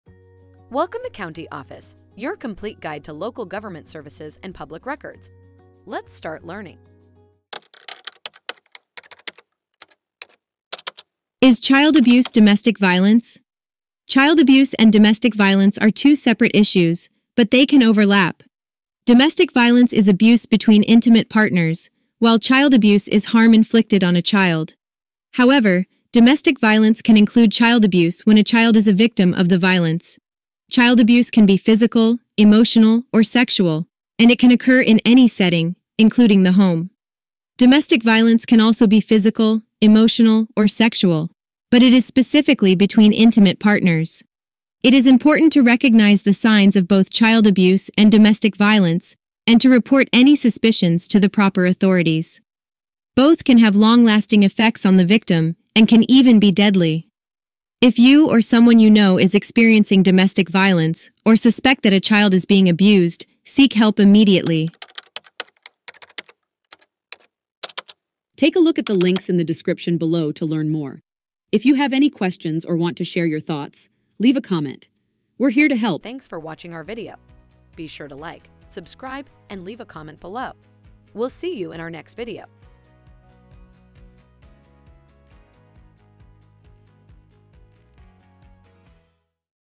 Is Child Abuse Domestic Violence? - CountyOffice.org
Is Child Abuse Domestic Violence? Are child abuse and domestic violence the same thing? Not exactly. In this eye-opening video, we'll break down the differences between child abuse and domestic violence, and how they can overlap.

Child abuse is when harm is inflicted on a child, whether it be physical, emotional, or sexual. Domestic violence, on the other hand, is abuse between intimate partners. However, domestic violence can also include child abuse when a child is a victim of the violence.

It's important to recognize the signs of both child abuse and domestic violence, and to report any suspicions to the proper authorities. Both can have long-lasting effects on the victim and can even be deadly.

In this video, we'll provide helpful resources for those who may be experiencing domestic violence or suspect that a child is being abused. We'll also discuss how to obtain help and support for victims.